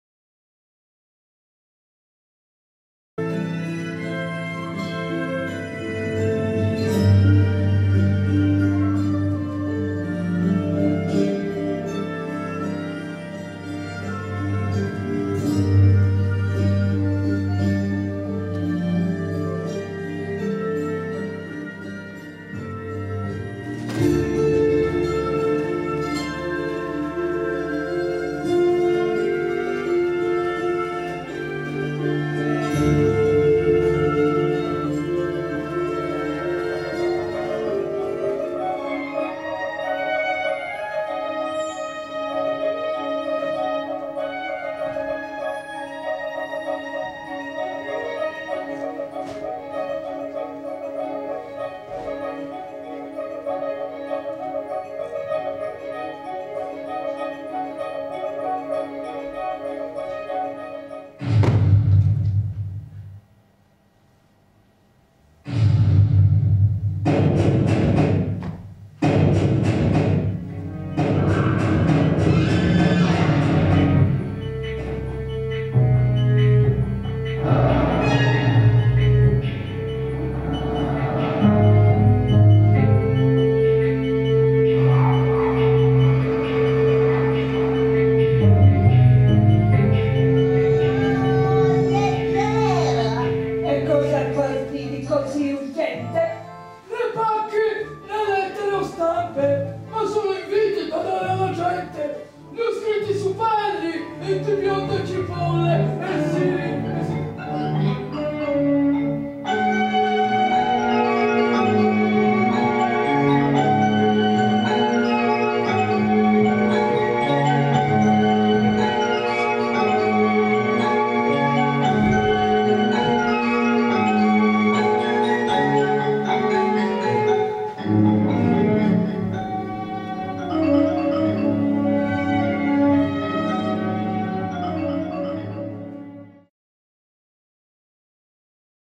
Serata in Nero - Teatro Verdi Milano - Dal 20 al31 ottobre 2010
Il "teatro su nero", una variante del teatro di figura, adotta l'uso di forme e figure animate mosse entro luci particolari da animatori camuffati nell'oscurità del palcoscenico; tali da creare suggestive e inaspettate apparizioni in grado di appassionare anche lo spettatore più scettico. Il Teatro del Buratto all'interno del panorama italiano si pone come eccellenza nell'uso di questa tecnica al punto da essere più volte presente sulla scena internazionale in occasione di rassegne teatrali su questo tema.